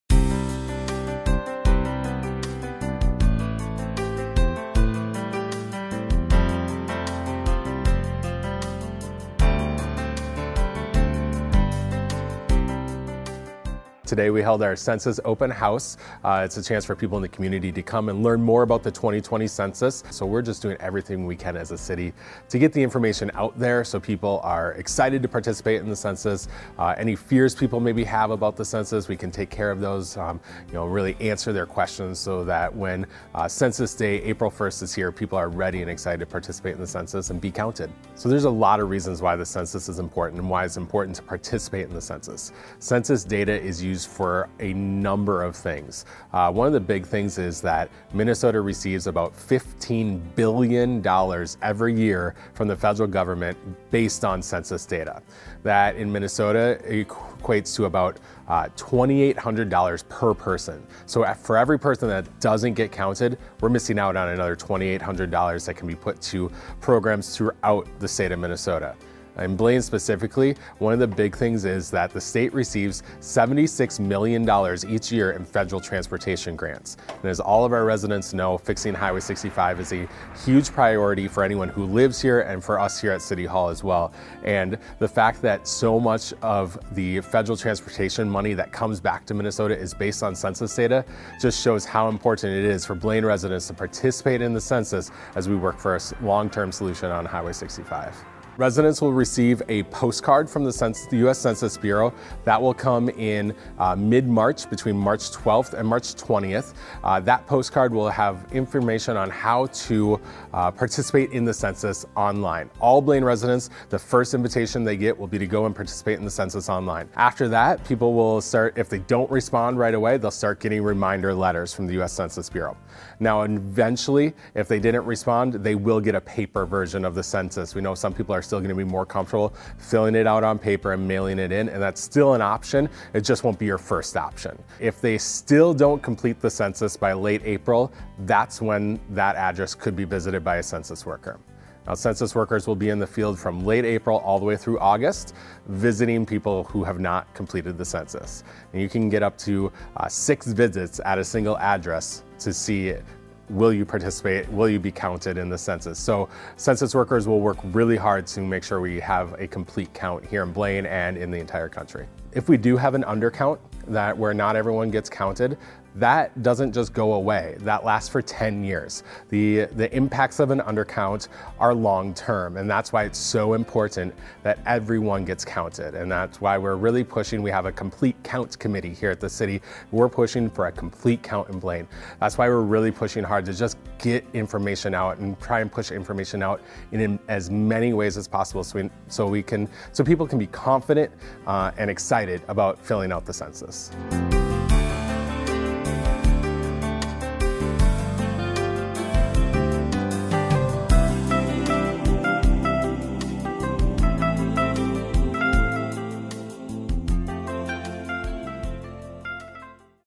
2020 Census: We Count BlaineMN
The 2020 will help set the course for the next decade in Blaine. Learn more about why participating in the census is so important.